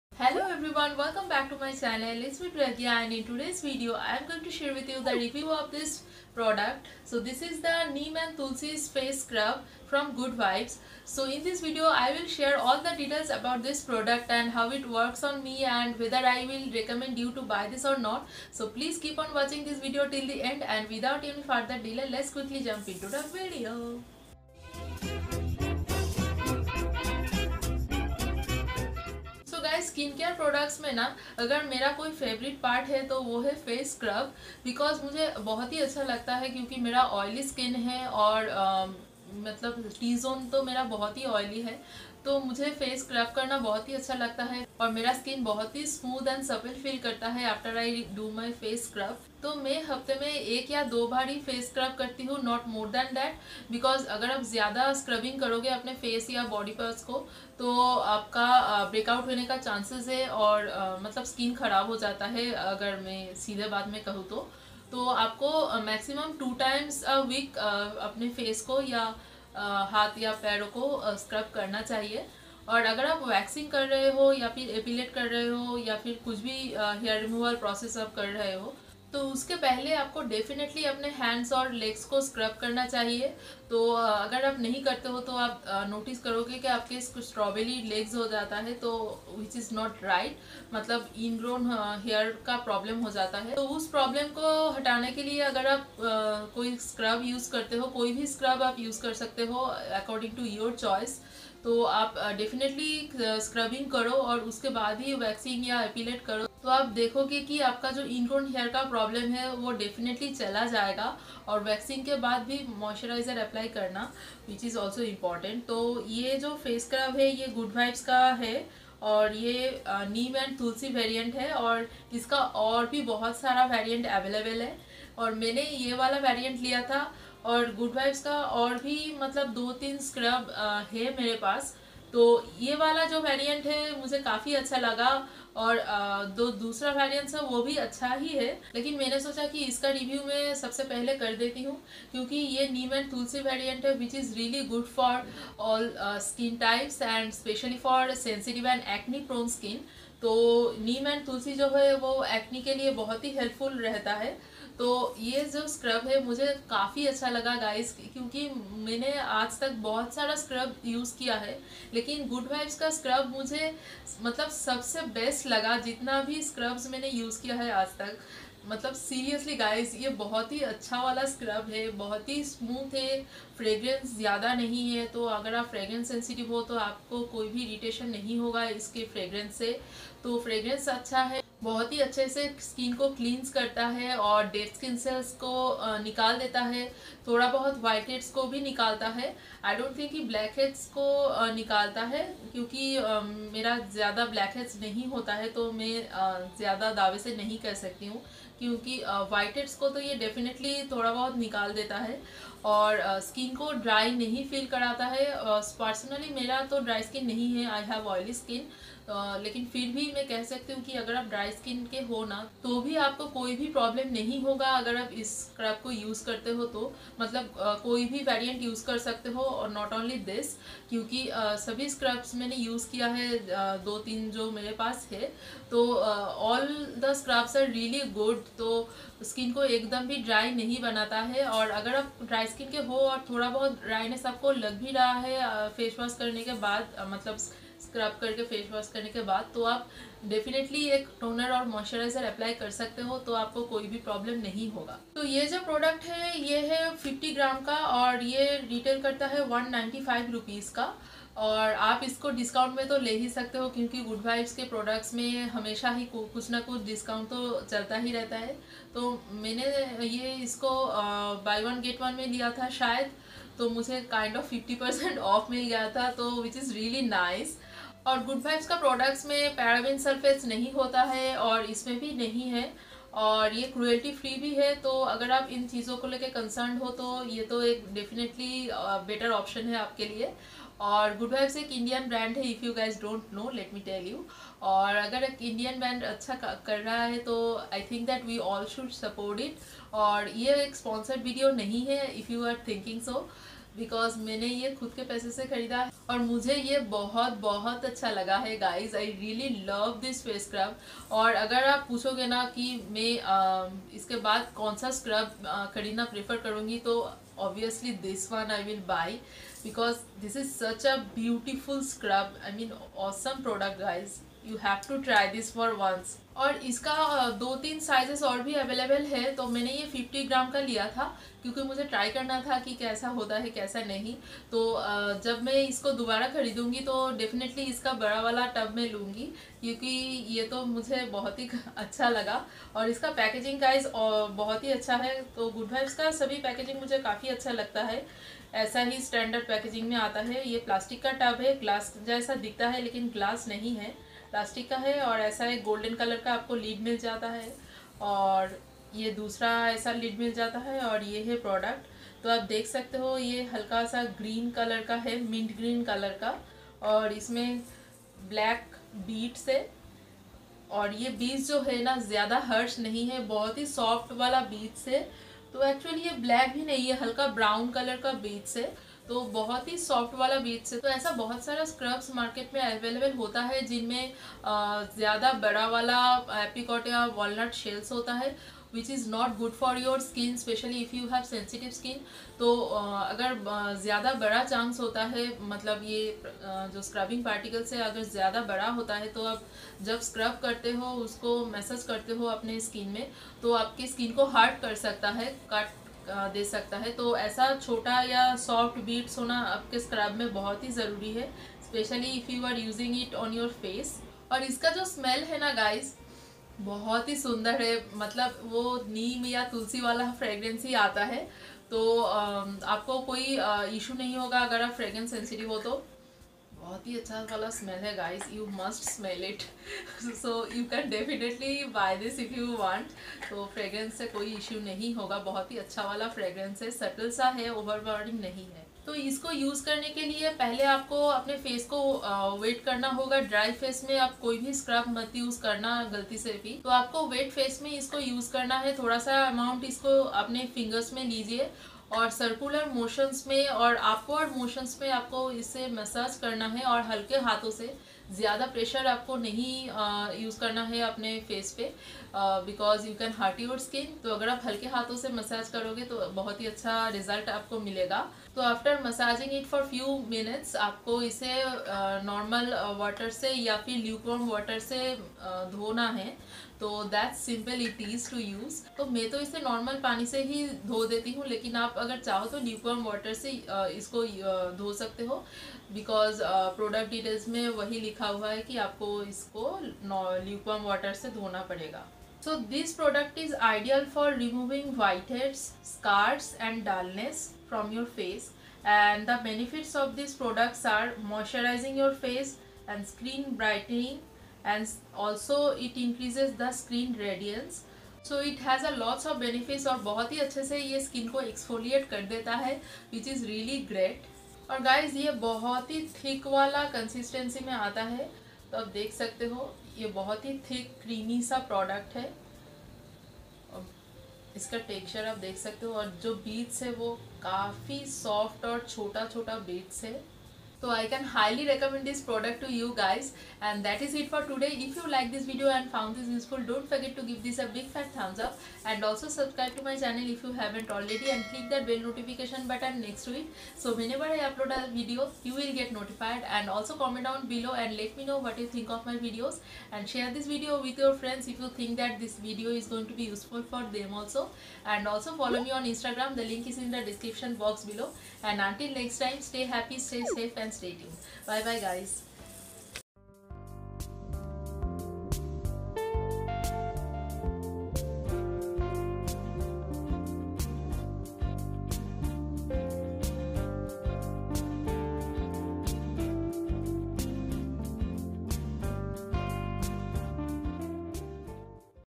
Best Scrub In Budget ? Honest Review Of GOOD VIBES Neem & Tulsi Purifying Face Scrub
Hey Everyone,
In today's video I am sharing my honest opinion about this GOOD VIBES Neem & Tulsi Purifying Face Scrub. Watch the video to know if I like it or not.

Product Link:
GOOD VIBES Neem & Tulsi Purifying Face Scrub

XOXO
Pragnya